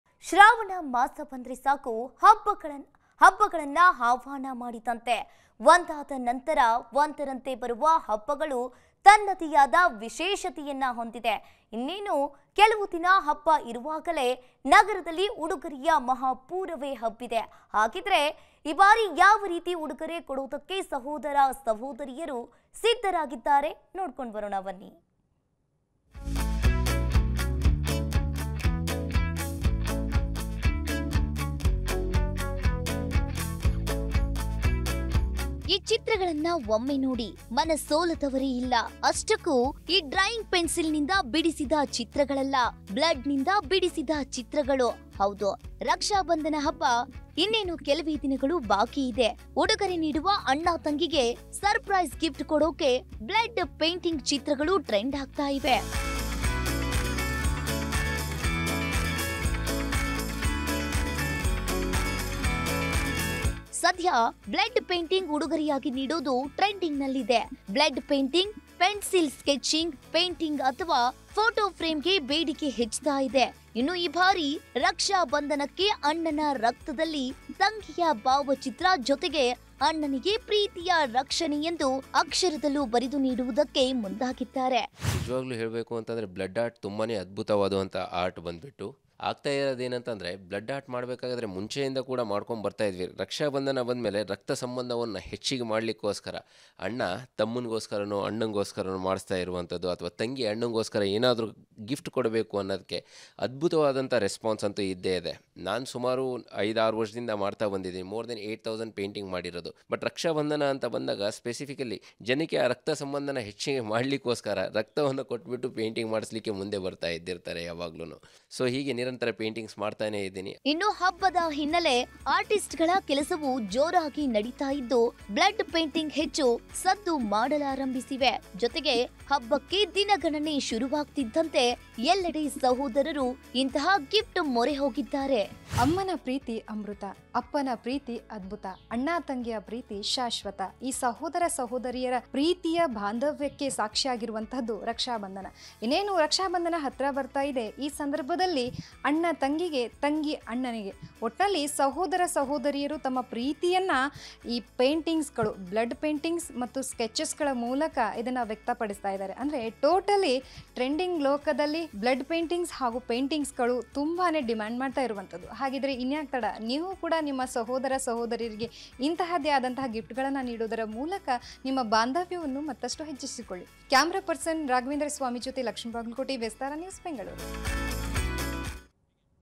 Blood art painting bhari demand vistara news kannada live Best blood painting Artist Bangalore
Blood art painting bhari demand vistara news kannada live
Best blood painting Artist Sanjeev Sangme Bangalore